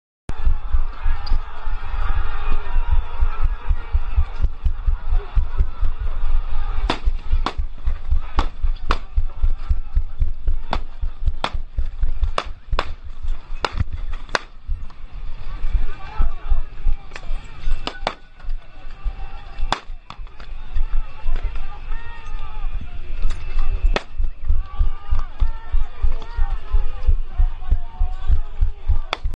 Joven cae por el impacto de un proyectil durante protesta en Altamira
La cámara portatil de un manifestante logró capturar el instante en el que cayó al pavimiento Fabián Urbina, de 17 años, quien poco después falleció en un centro asistencial de Chacao. Testigos denuncian que funcionarios de la GNB usaron sus armas de fuego para dispersar la protesta